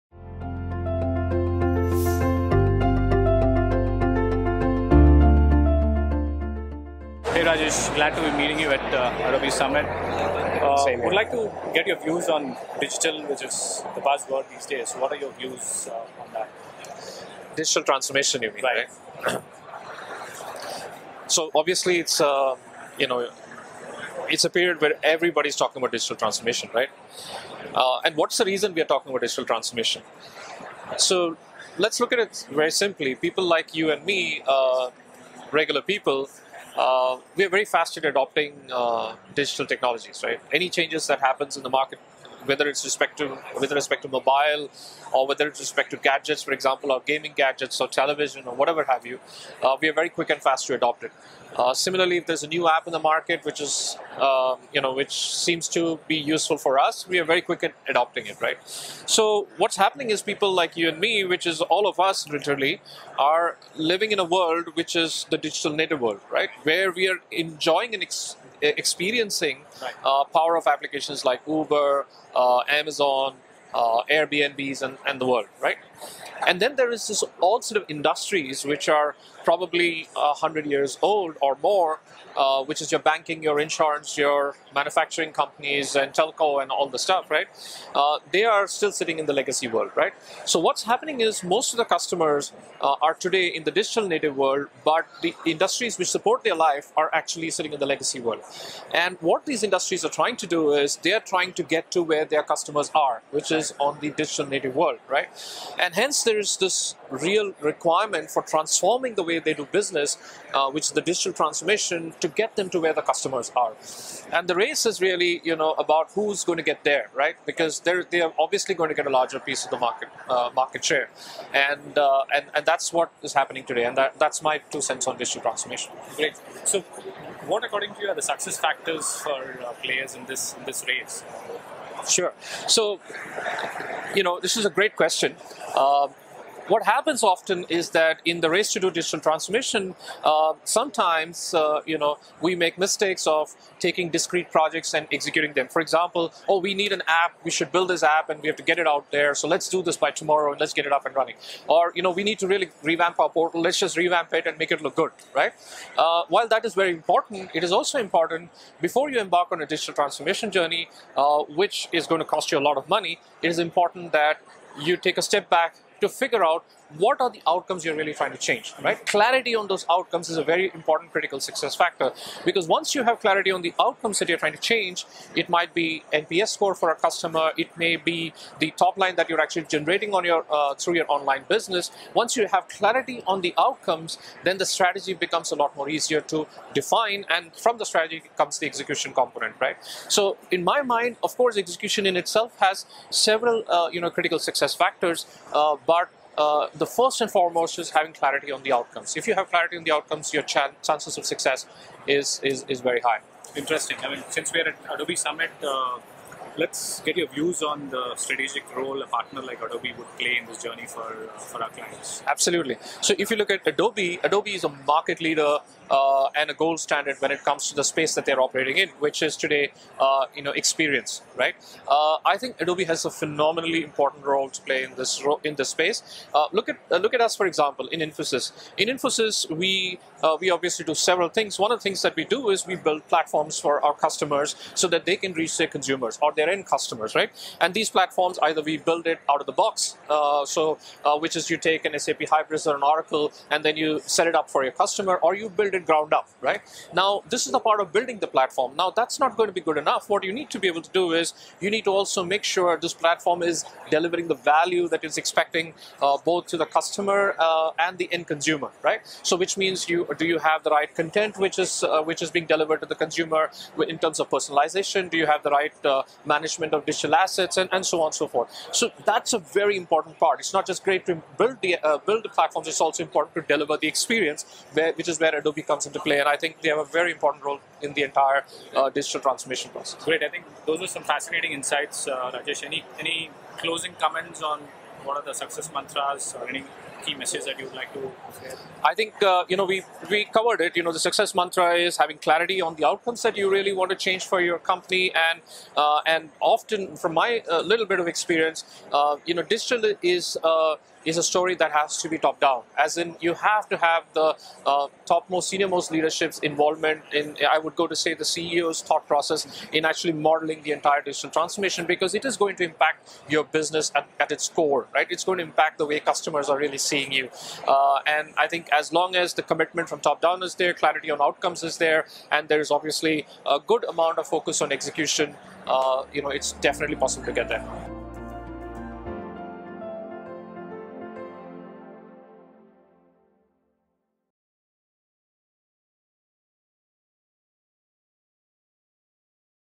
Ingredients of a Successful Digital Transformation
Rajesh Varrier, Senior Vice President and Head of Microsoft and Digital Experience, Infosys, explains how digital is a pathway that can bring legacy companies closer to their digital native customers. He shares his insights on how to succeed in the digital transformation journey with an outcome based approach.